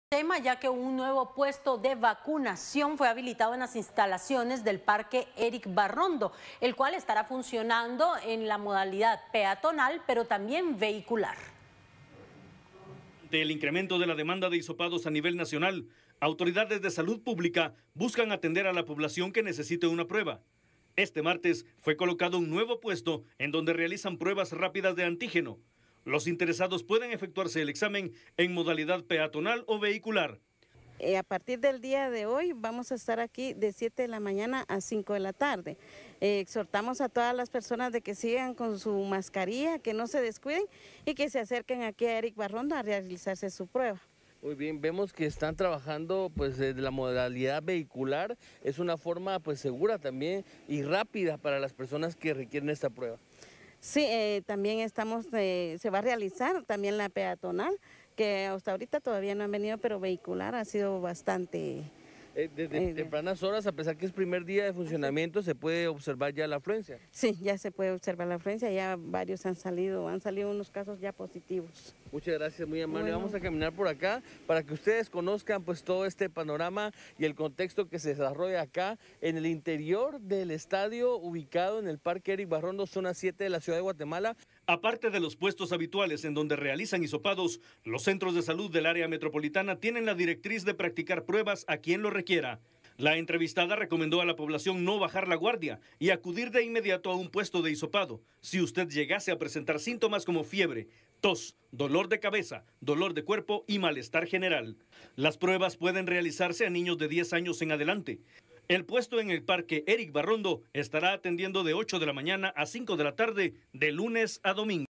Habilitan nuevo puesto de hisopados en el estadio Erick Barrondo
Se reportan un alto índice de positividad

Derivado del reunte de casos de COVID-19 autoridades de salud decidieron habilitar las instalaciones en el parque Erick Barrondo para la realización de hisopados.